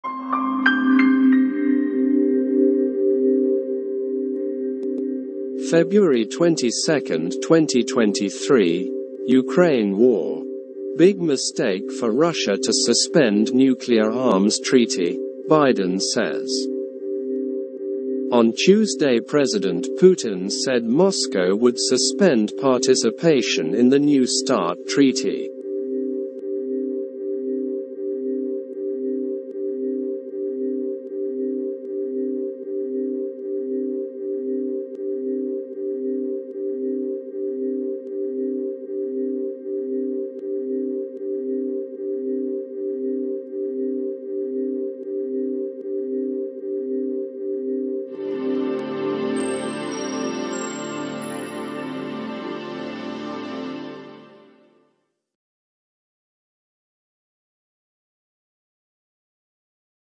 [Latest]Ukraine war: Big mistake for Russia to suspend nuclear arms treaty, Biden says
[Publication Date]

[Contents]
On Tuesday President Putin said Moscow would suspend participation in the New Start treaty.